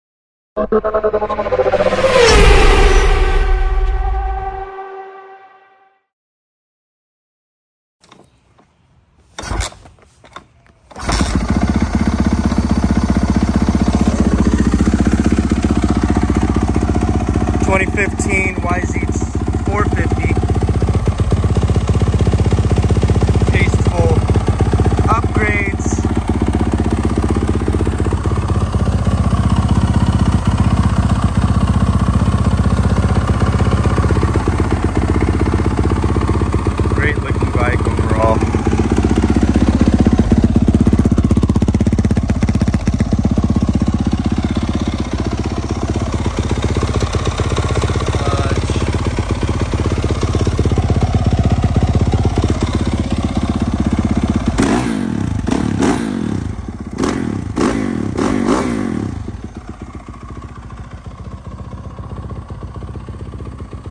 2015 Yamaha YZ450F | Motor-Speed
Gorgeous bike for sale here at Motor-Speed LLC!

2015 YZ450F 🏁
-5 hours on build
-Fuel Injected
-Tasteful Upgrades

Asking only $4,995!